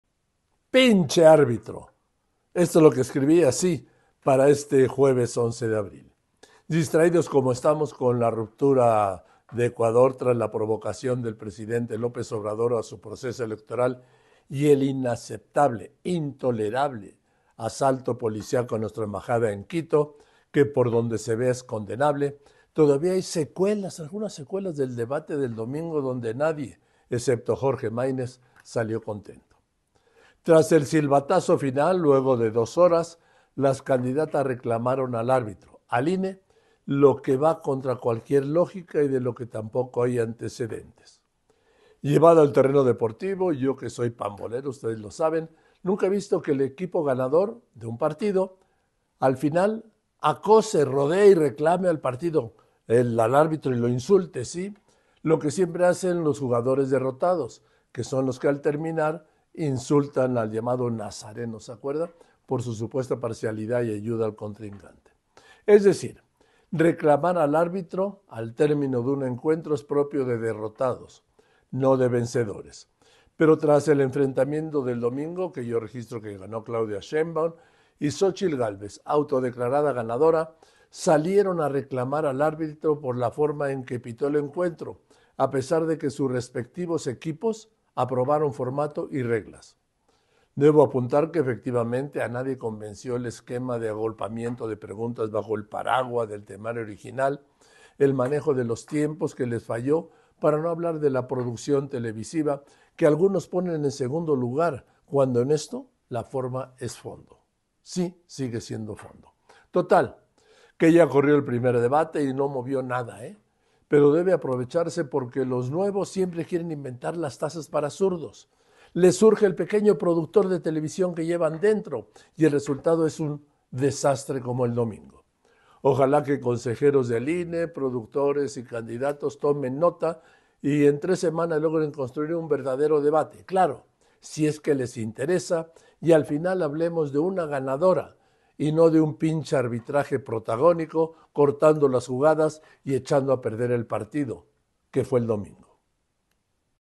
Pinche árbitro
Ojalá que consejeros del INE, productores y candidatos, tomen nota y en tres semanas logren construir un verdadero debate, si es que les interesa, claro, y al final hablemos de una ganadora y no de un pinche arbitraje, protagónico, cortando las jugadas y echando a perder el partido.